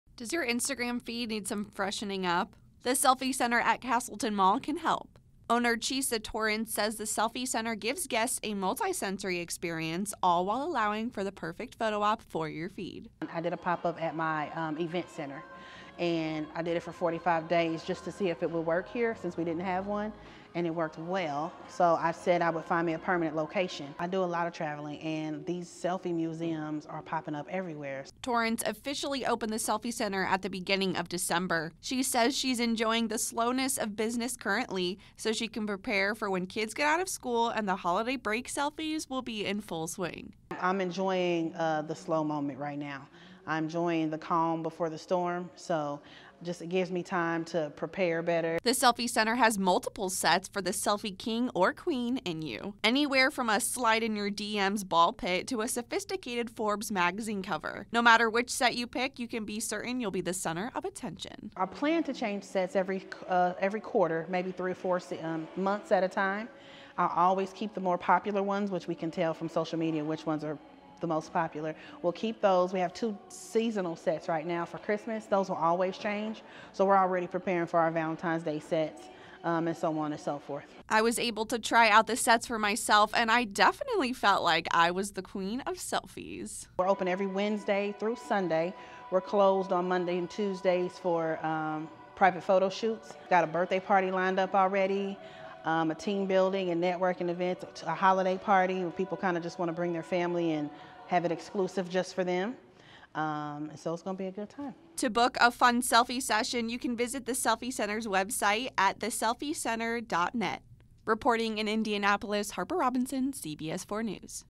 New Selfie Museum Castleton mall
Need some new Instagram content? Look no further than Castleton mall's new Selfie Centre.